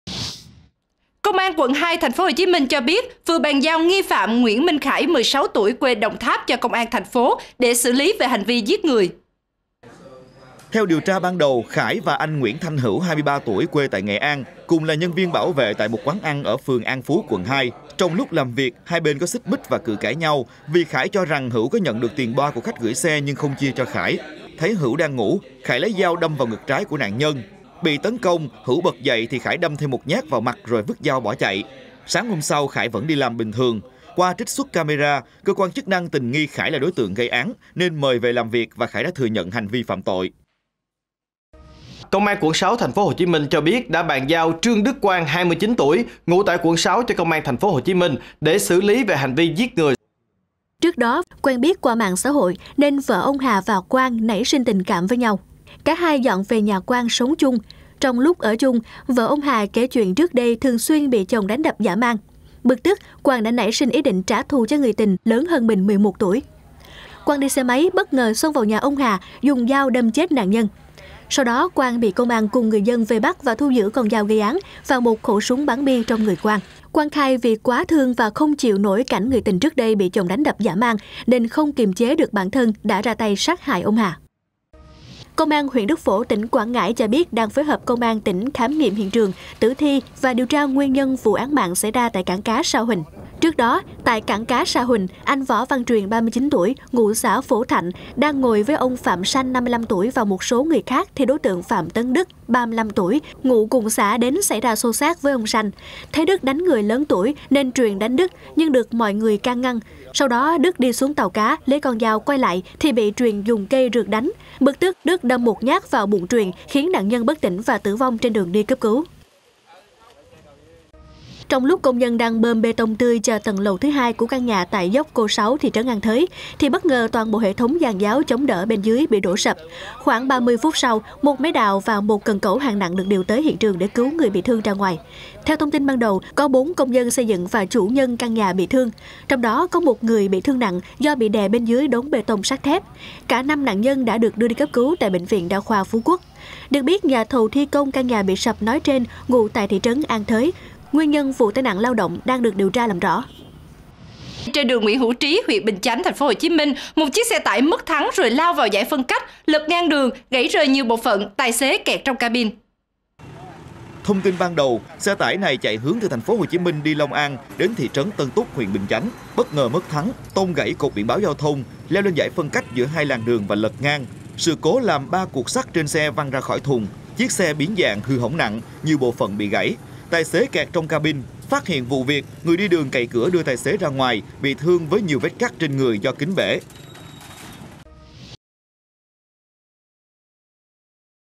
Giành tiền boa, BẢO VỆ 16 TUỔI đâm chết đồng nghiệp | Toàn cảnh 24h
K. và anh H. xảy ra cự cãi dẫn đến xô xát vì tranh giành tiền bo. Trong lúc giằng co, anh H. có đánh một cái vào đầu K. Ấm ức, K. bỏ việc về trước.

K. nảy sinh ý định đâm anh H. bị thương để anh này bỏ việc nên quay lại quán ăn. Lúc này, thấy anh H. đang ngủ, K. xuống bếp cầm dao đâm 1 nhát trúng ngực anh H.

► Đăng ký để xem nhiều hơn: goo.gl/5rGxjy